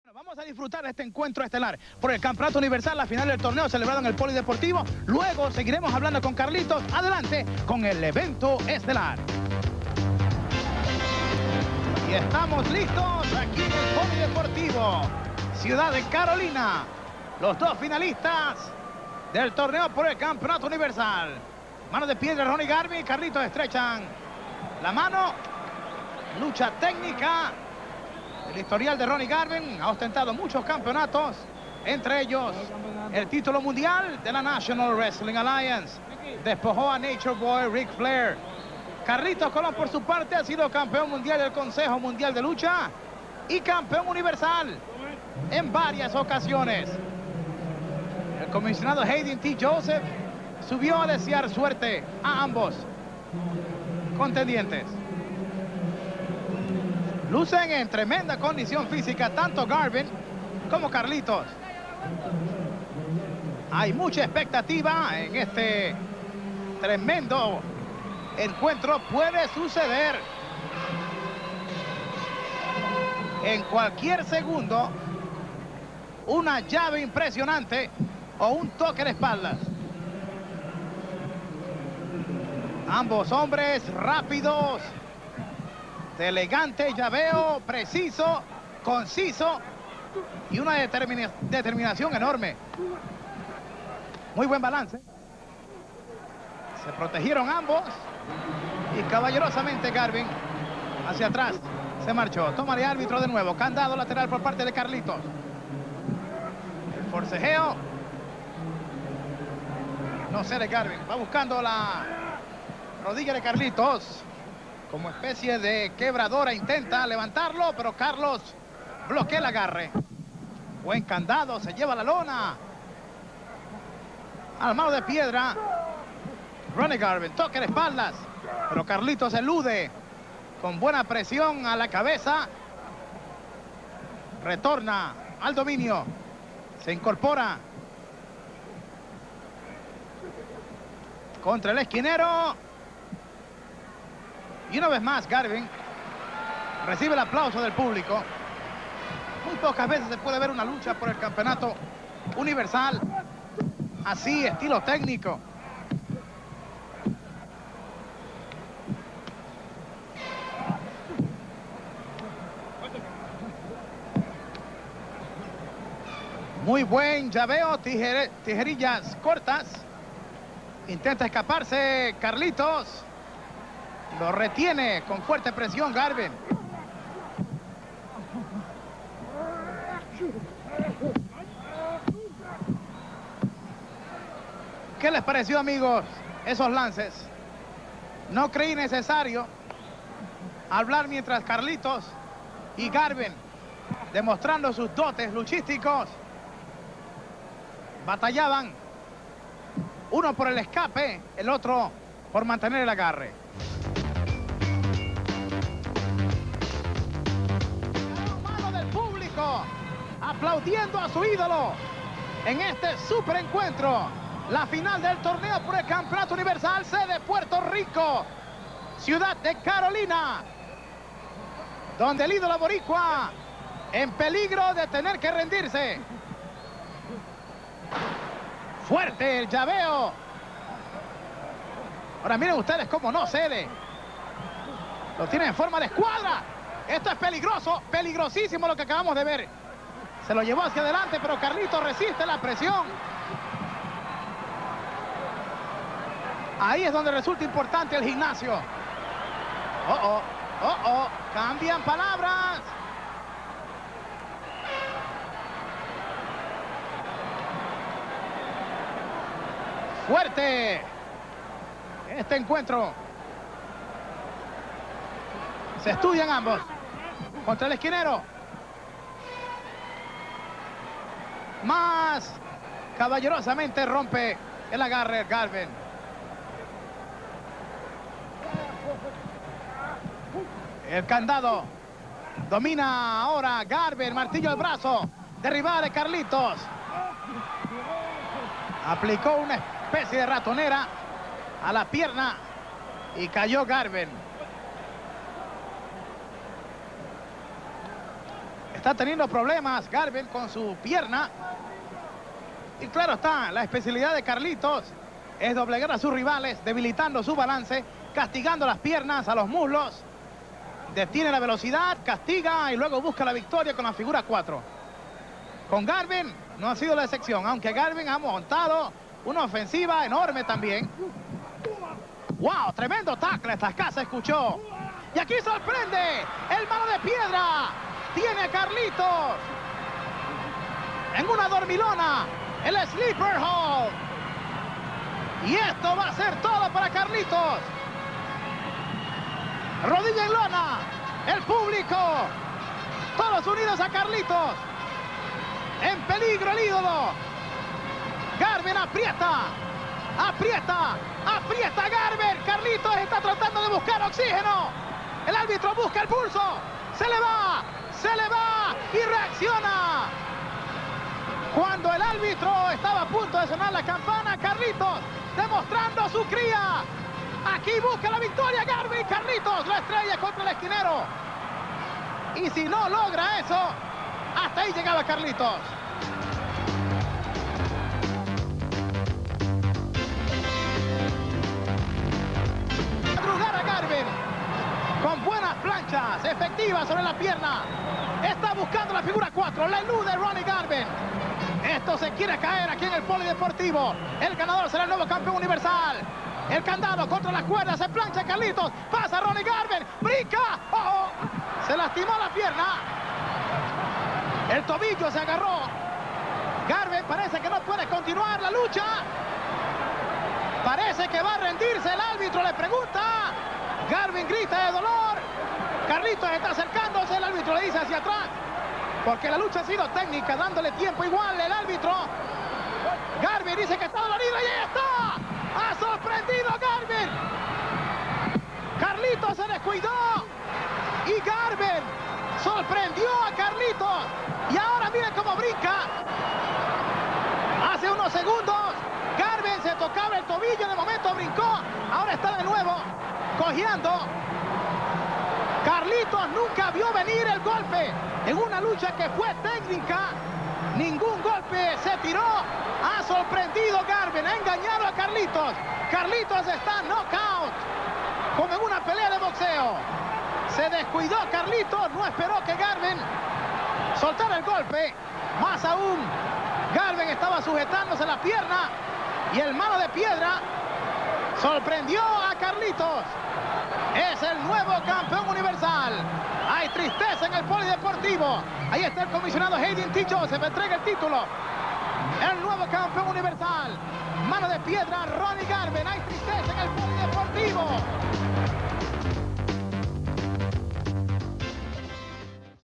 WWC 1988 Carlos Colón VS Ronnie Garvin Nov 24 1988
Carlos Colón frente a Ronnie Garvin. Final del torneo por el título Universal. 24 de noviembre de 1988.